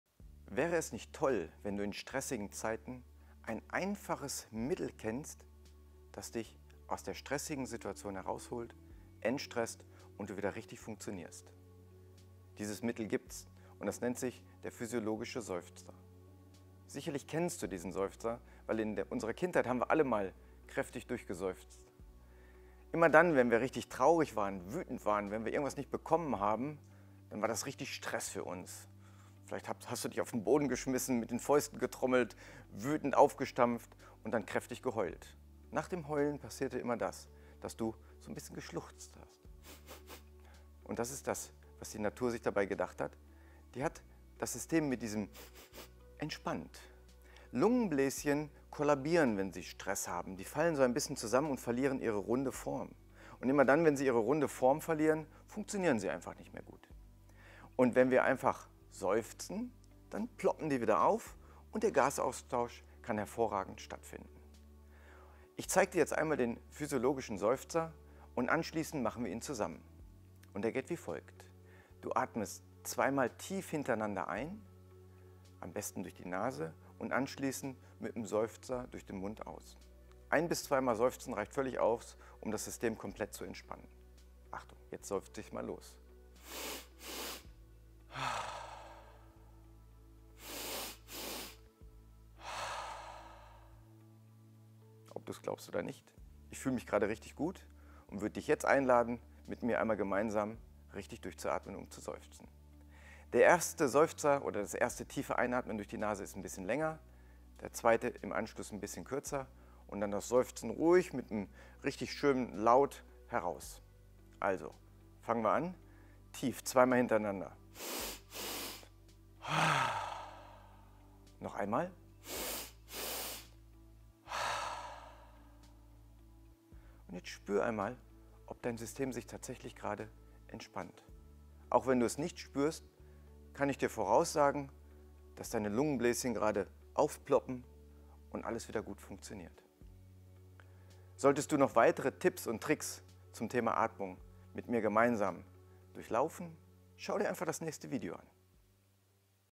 Besser Atmen im Alltag #03 | Entstressung durch physiologischen Seufzer | ARTZT neuro
Dirk Schauenberg von Fitnessland Kensho zeigt dir ein einfaches Mittel, das dich aus der Stressfalle holt.